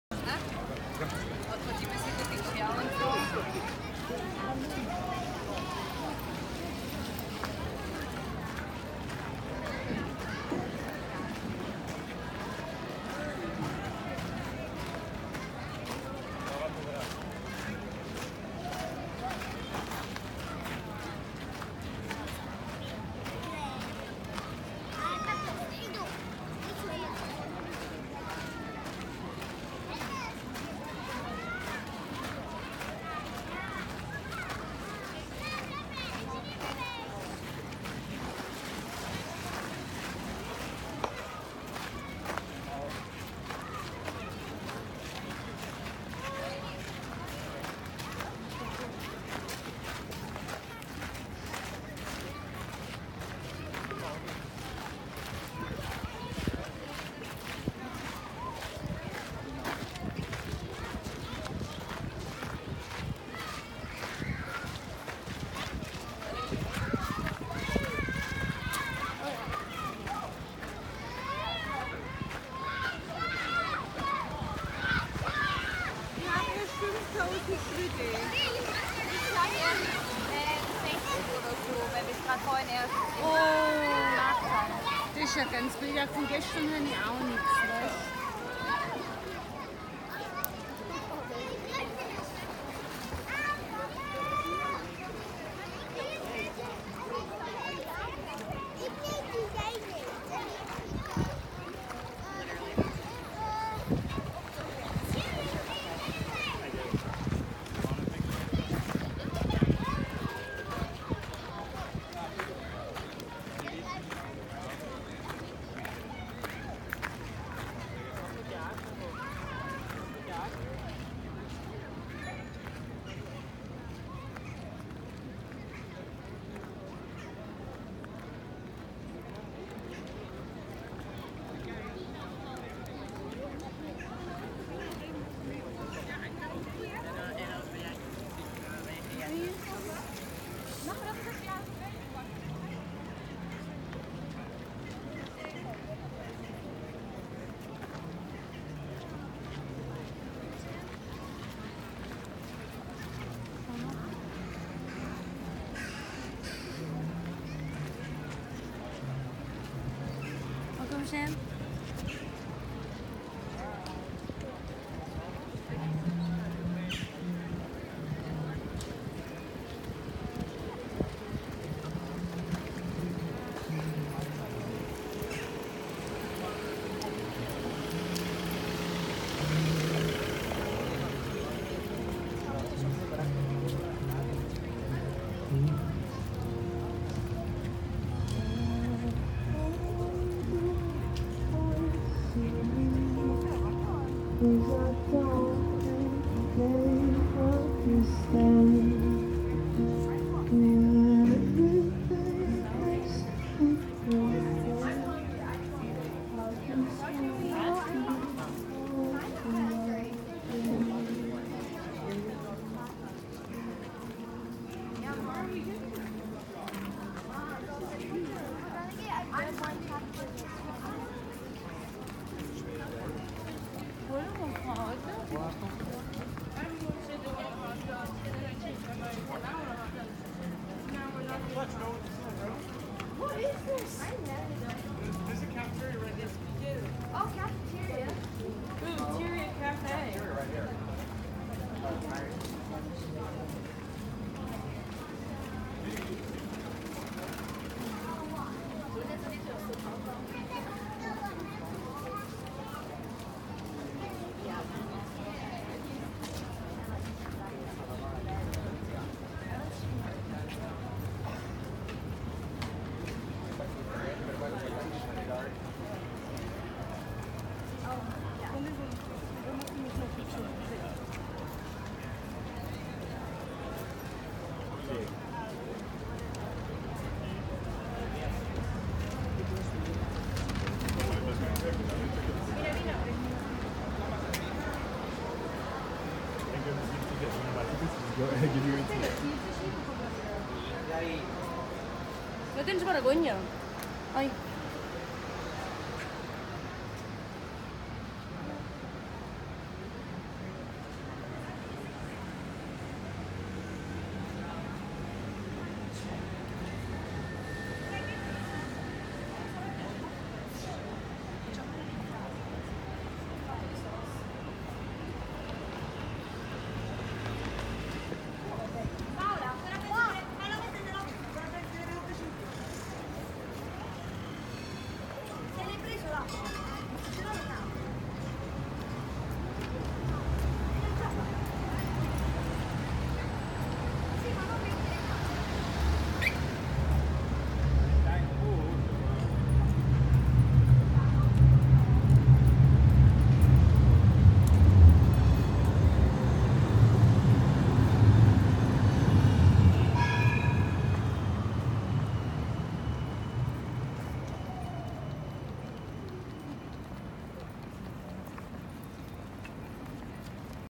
Walk around Art Square Amsterdam
Short walk around the pool of Art Square Amsterdam